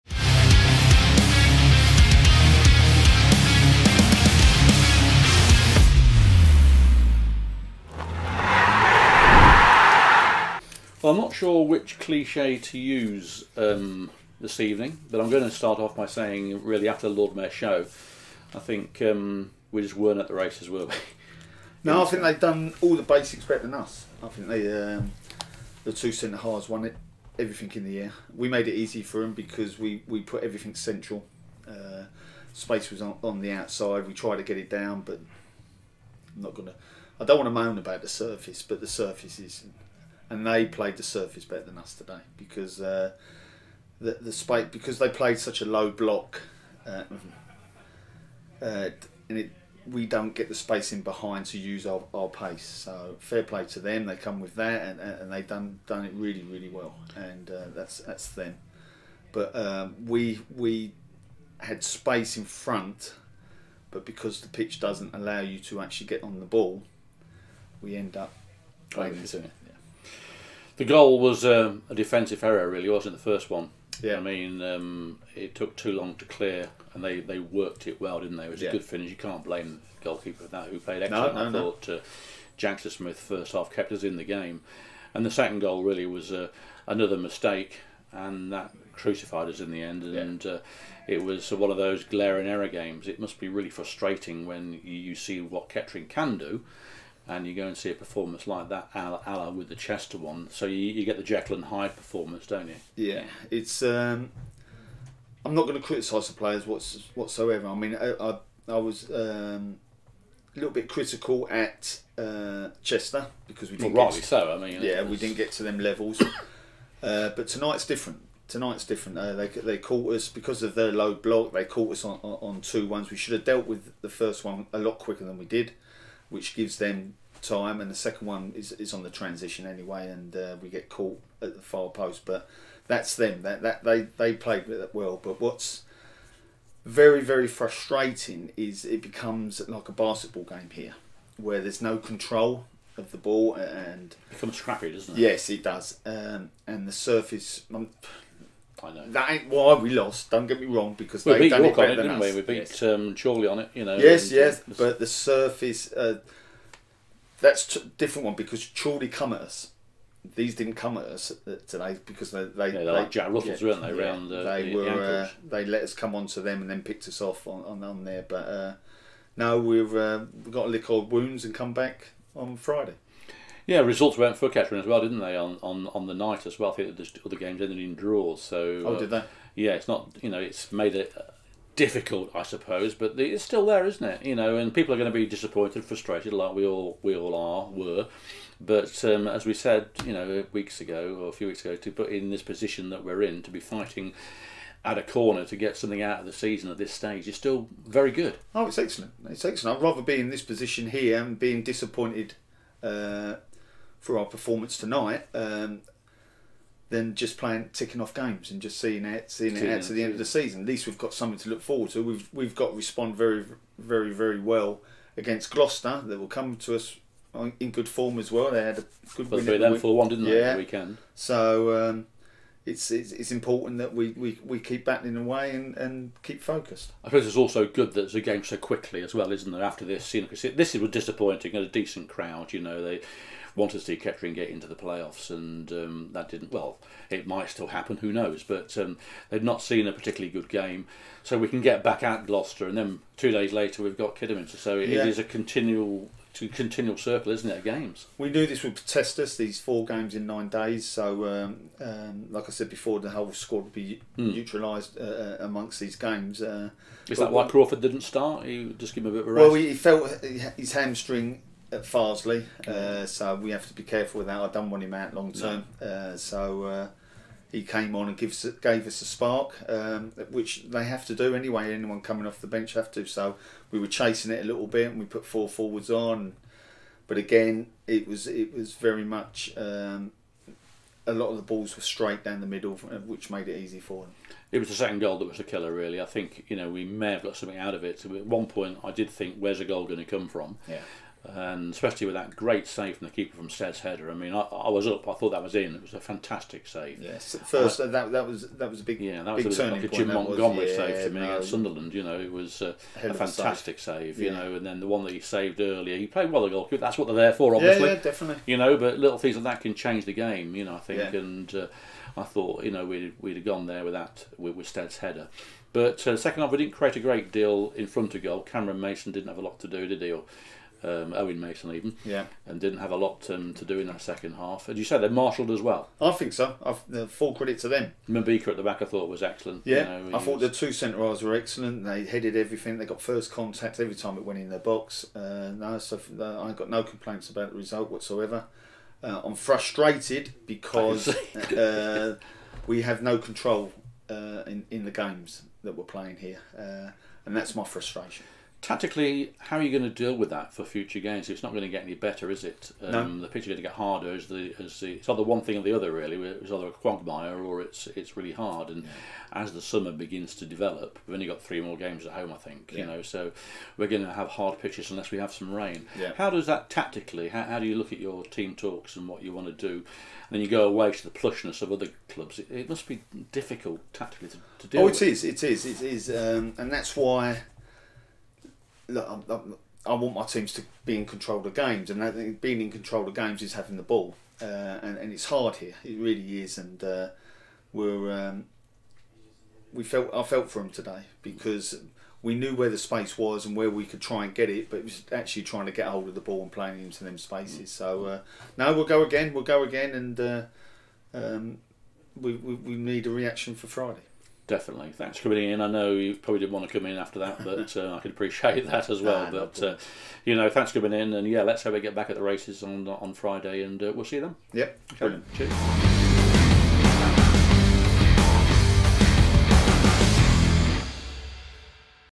Ian Culverhouse Interview - Guiseley - 12/04/2022
Paul Cooke speaks to Ian Culverhouse following Kettering Town's 2-0 defeat at home to Guiseley in the Vanarama National League North.